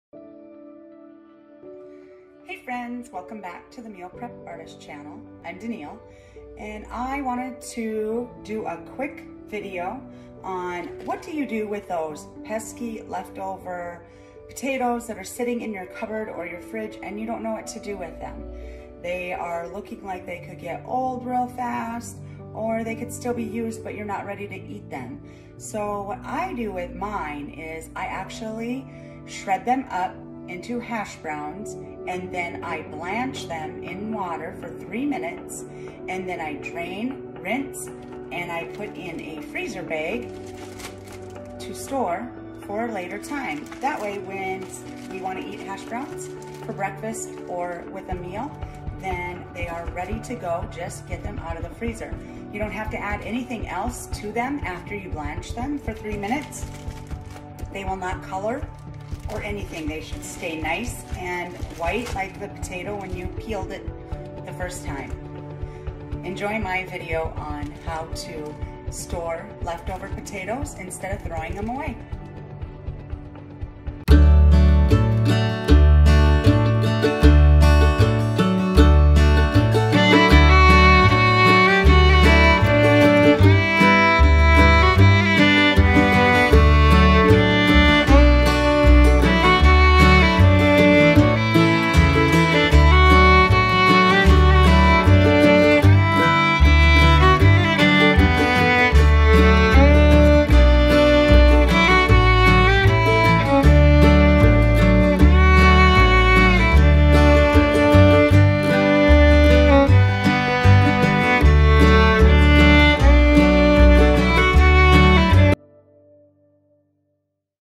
Shredded Potatoes - Saves you money two ways!
Shredding your left over unused potatoes instead of throwing them away.
Save money in two ways.
1 - use your left over potatoes instead of throwing them away
AKA: throwing your money away.
2 - shred those potatoes to make your own homemade hashbrowns
AKA: saving you money from buying store processed hashbrowns.
Watch this 2 minute kitchen hack and enjoy watching your savings account grow!

TicTok - dyneilmo